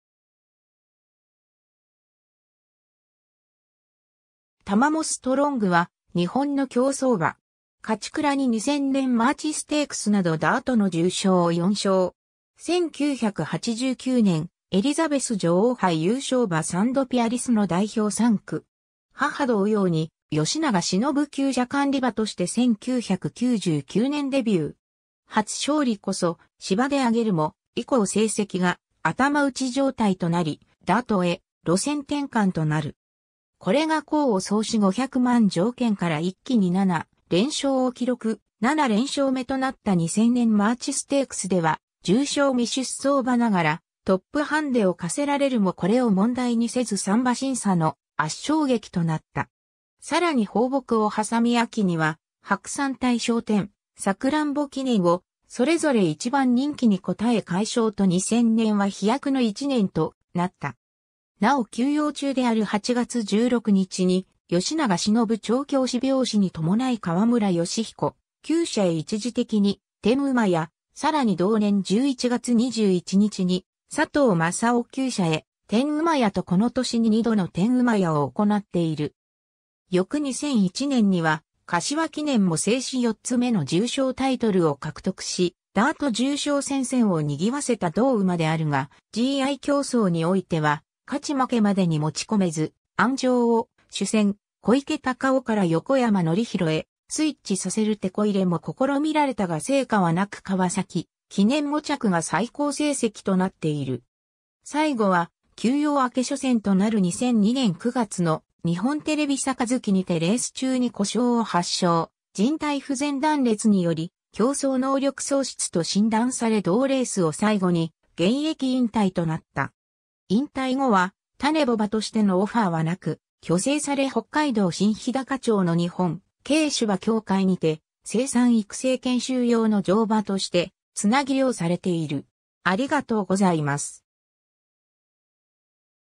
タマモストロング
タマモストロングは日本の競走馬。
勝ち鞍に2000年マーチステークスなどダートの重賞4勝。
1989年エリザベス女王杯優勝馬サンドピアリスの代表産駒。
 母同様に吉永忍厩舎管理馬として1999年デビュー。
初勝利こそ芝で挙げるも、以降成績が頭打ち状態となりダートへ路線転換となる。
 これが功を奏し500万条件から一気に7連勝を記録、7連勝目となった2000年マーチステークスでは重賞未出走馬ながらトップハンデを課せられるもこれを問題にせず3馬身差の圧勝劇となった。
さらに放牧を挟み秋には白山大賞典・さくらんぼ記念をそれぞれ1番人気に応え快勝と2000年は飛躍の1年となった。
 なお休養中である8月16日に吉永忍調教師病死に伴い川村禎彦厩舎へ一時的に転厩、さらに同年11月21日に佐藤正雄厩舎へ転厩とこの年に2度の転厩を行っている。
 翌2001年にはかしわ記念も制し4つ目の重賞タイトルを獲得しダート重賞戦線を賑わせた同馬であるが、GI競走に於いては勝ち負けまでに持ち込めず、鞍上を主戦小池隆生から横山典弘へスイッチさせるテコ入れも試みられたが成果は無く川崎記念5着が最高成績となっている。
 最後は休養明け初戦となる2002年9月の日本テレビ盃にてレース中に故障を発症、靭帯不全断裂により競走能力喪失と診断され同レースを最後に現役引退となった。
 引退後は種牡馬としてのオファーは無く、去勢され北海道新ひだか町の日本軽種馬協会にて生産育成研修用の乗馬として繋養されている。